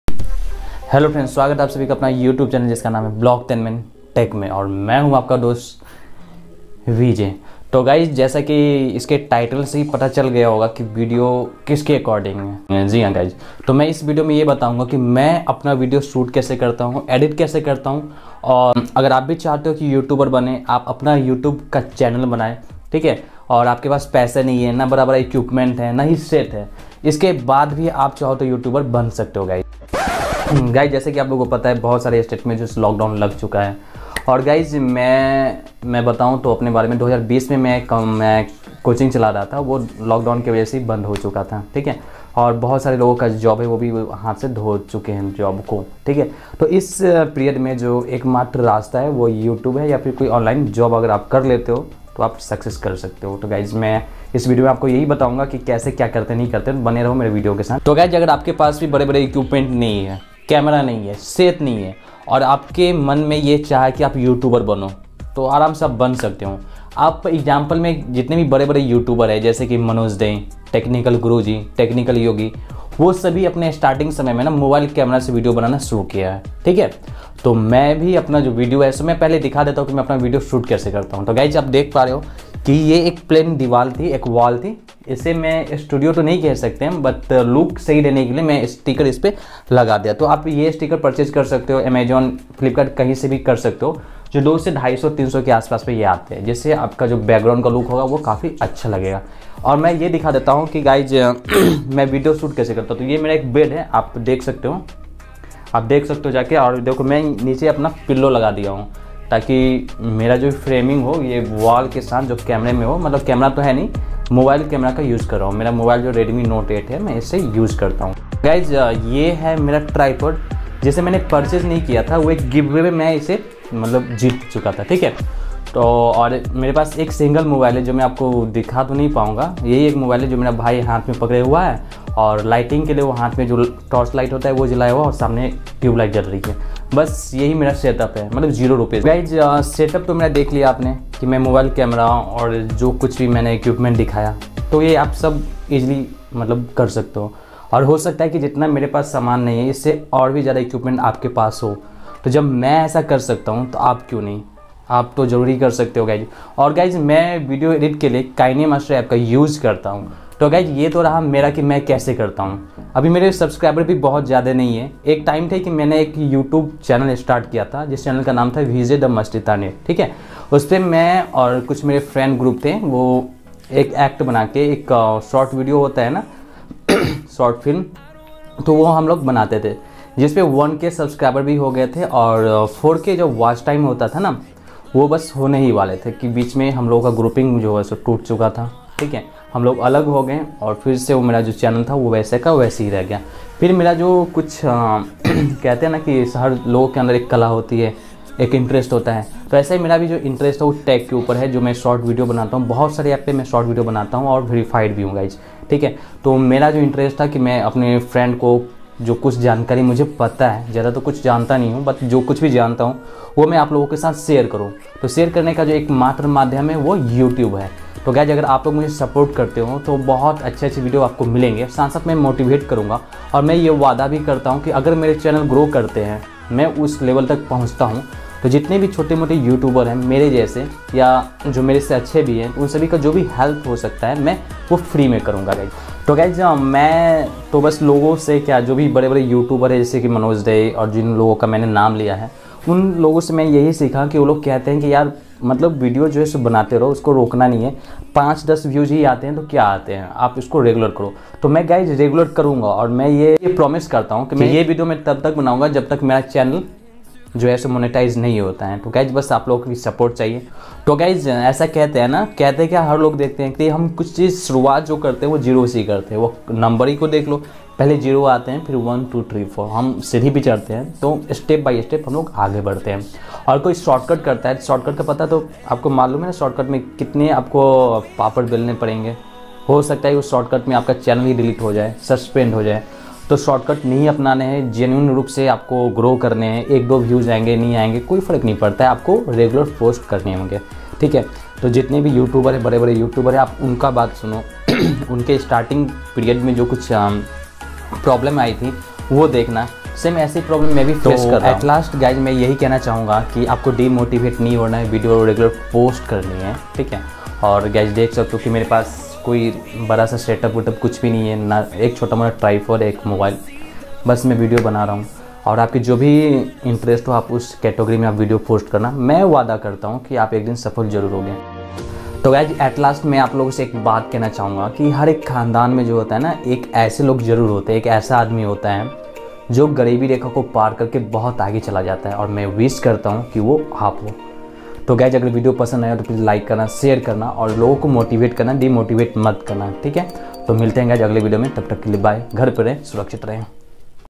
Youtuber kaise bane bina kisi mahange equipment ke | How to become a successful youtuber in 2021
By vlogtainmenttech
:- video content:-
•iss video  me ap janoge ki bina mahange saman camera set k bhi ap kaise youTuber ban sakte ho..
•YouTuber banne k kuch unique tips...
•mai apna youTube video kaise banata edit kaise karta sb kuch ap dekhoge iss video me
•Saath me dher sari motivational wali baate
To plz video last tak zarur dekhe nd mujhe support kare 🙏🥰.